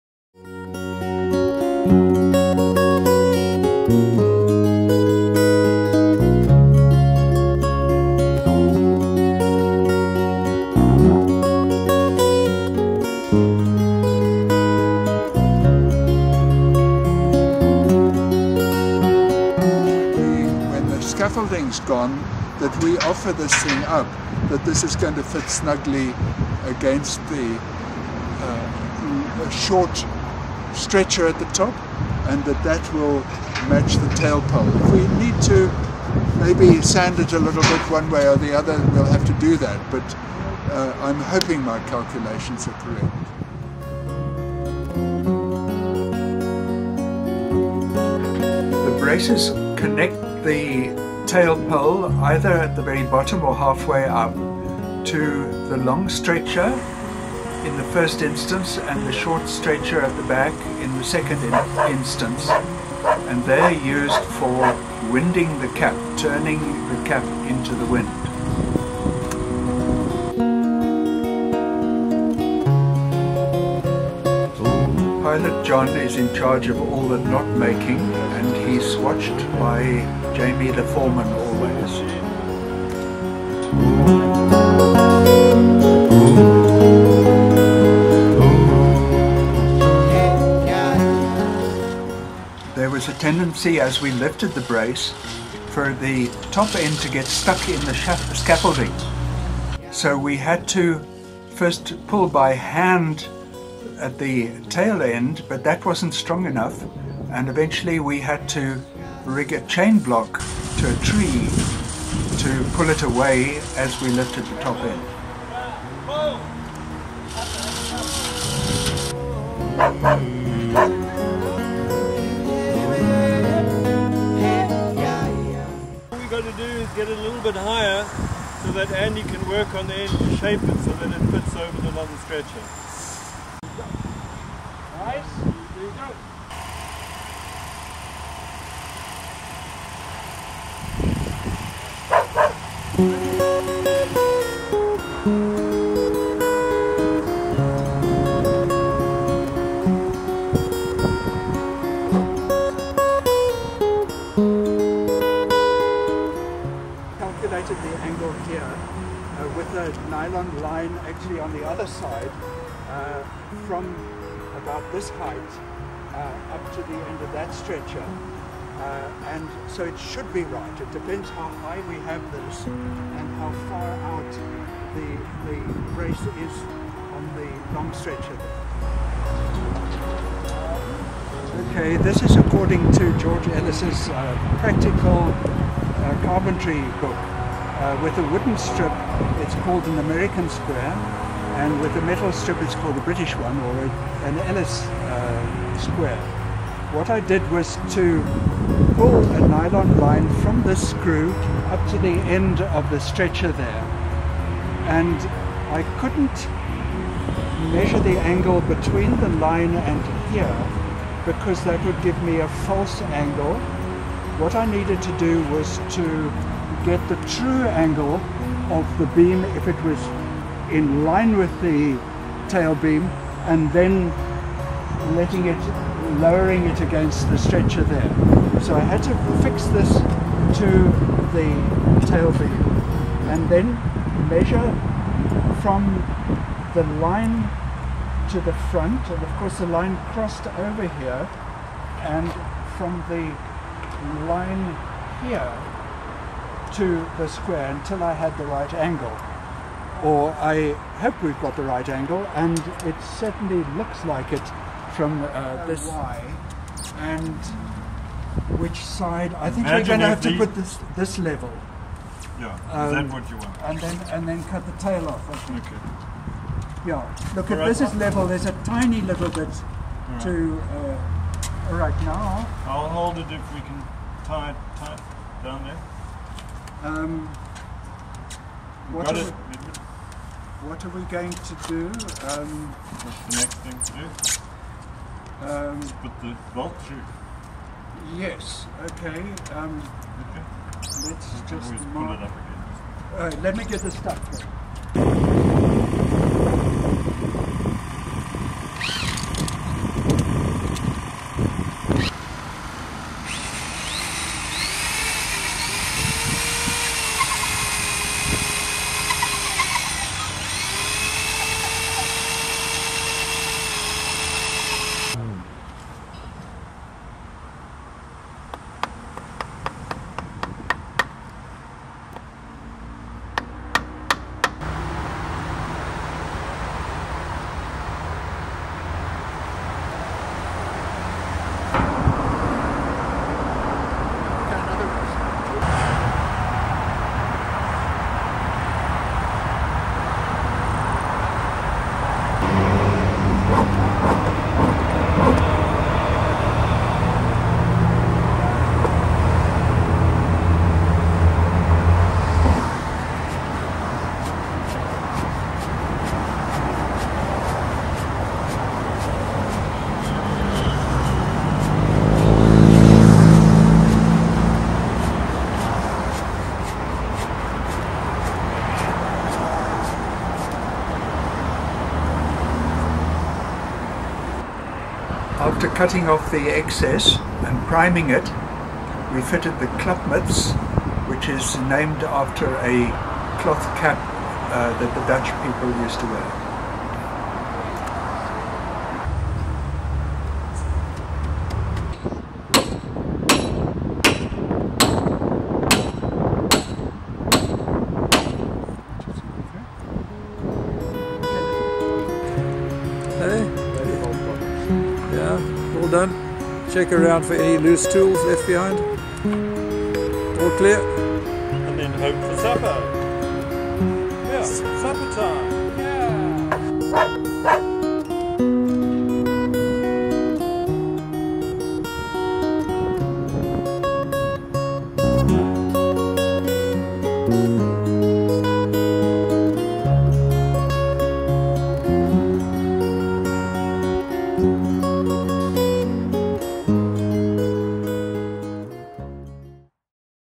Mostert's Mill Braces Part 6
Mostert's Mill is a 220 year old mill in Cape Town that burned down in April 2021.  With great effort from volunteers in Friends of Mostert's Mill and many contributions from donors and businesses the mill is almost ready to perform again.   Part six shows the erection of the braces.  December 2022